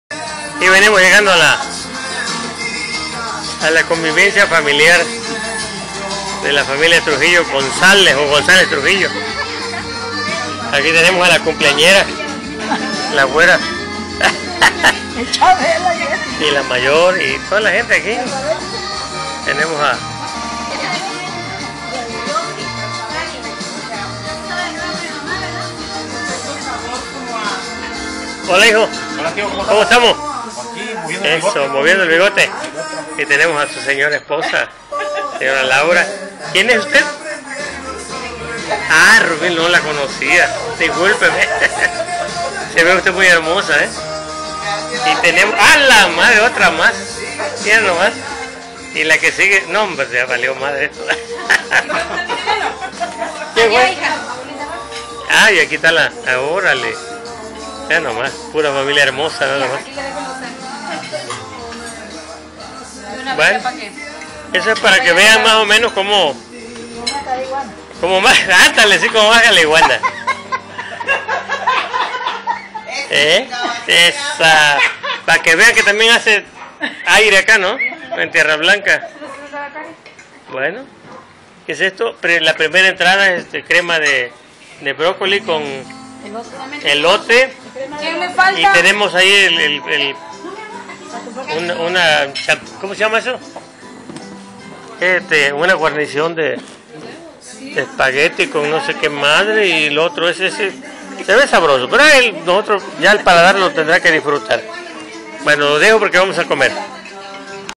Cumpleaños de bety
Comida en honor de mi sobrina Bety. González Trujillo. .en tierra Blanca Veracruz México